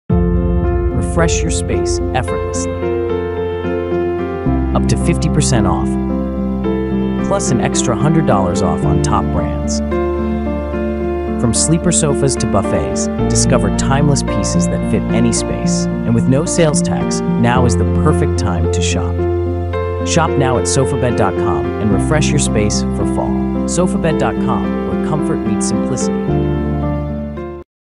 Fall Sale! Up to 50% Off + Extra $100 Off on Top Brand Furniture | Sofabed.com
Refresh your space effortlessly this fall with up to 50% off, plus an extra $100 off on top-brand furniture at Sofabed.com. From sleeper sofas to buffets, discover timeless pieces that fit any room, and enjoy no sales tax! Don't miss out—shop now and transform your home for the season. Visit Sofabed.com for comfort and simplicity.

Tags -